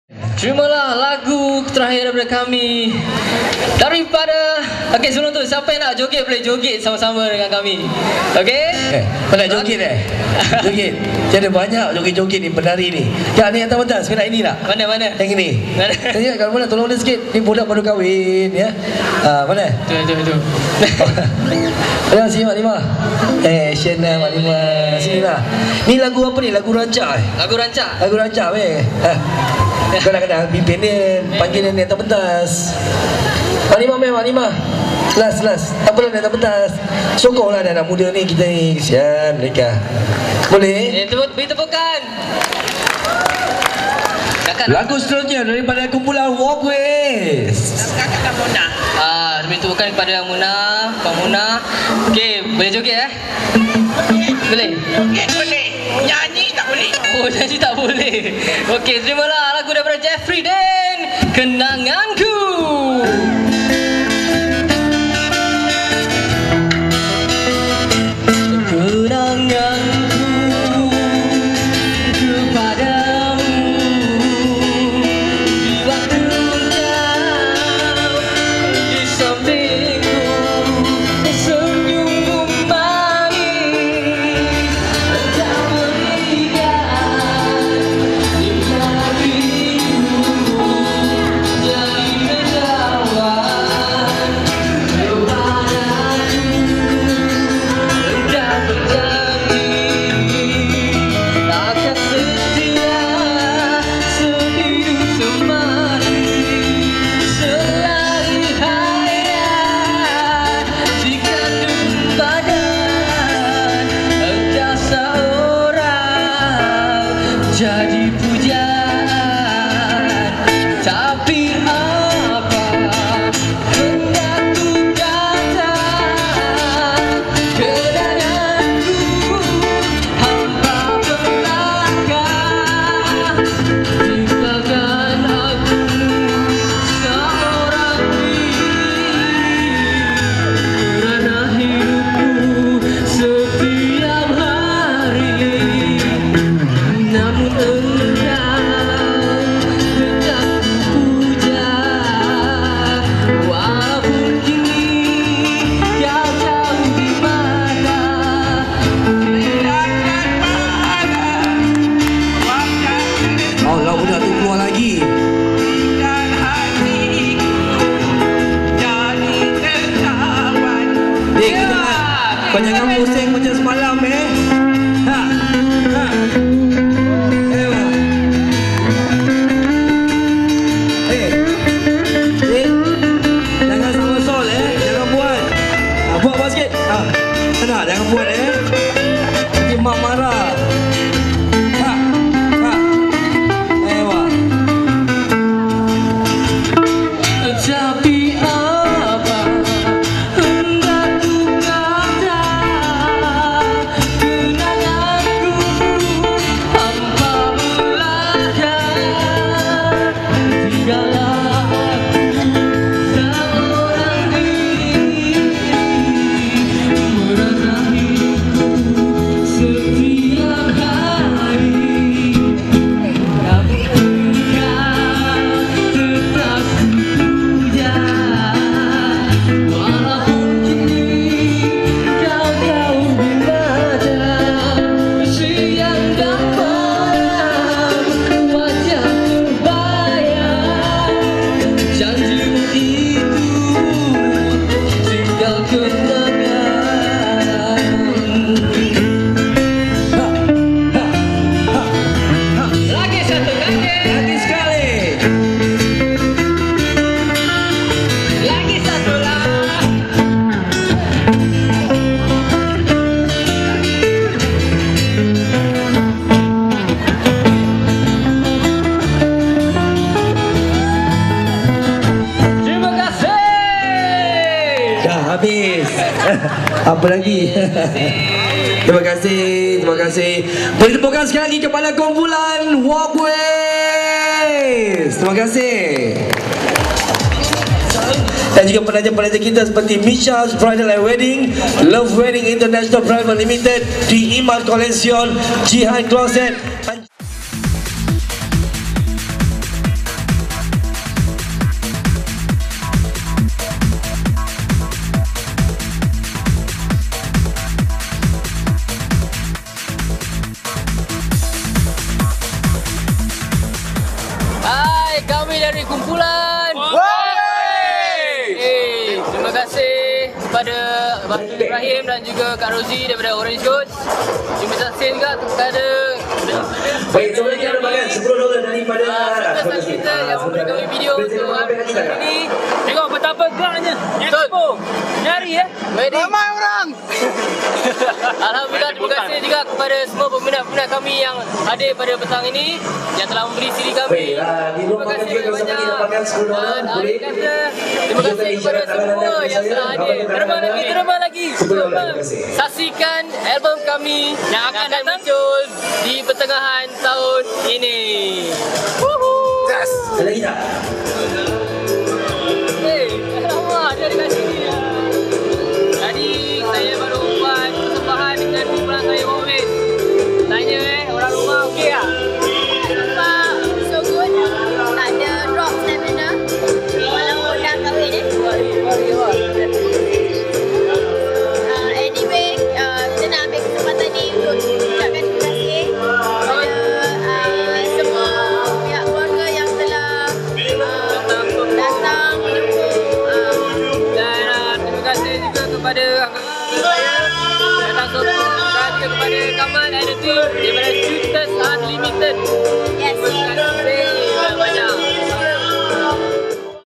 WALKWAYZ @ SG Expo Part 4 SHOOTERZ UNLIMITED.
Performance by: WALKWAYZ Band, Singapore Expo (Best of Bridal Lifestyle & Fiesta Indonesia 2012 by Orange Private Limited).
Video Production: Shooterz Unlimited, Kamal Matsonic